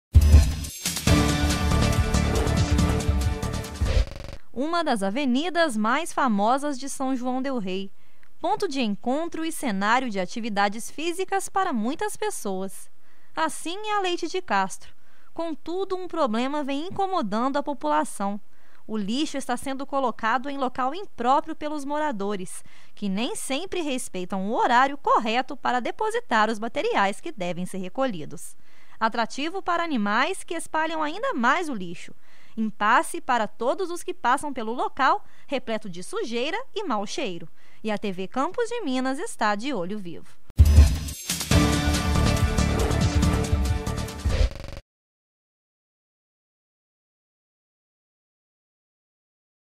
Vala na Avenida Leite de Castro Acumula Água Suja
Na Avenida Leite de Castro existe uma grande vala com água suja e parada. Segundo os moradores essa situação já se arrasta por anos. Confira no quadro Olho Vivo!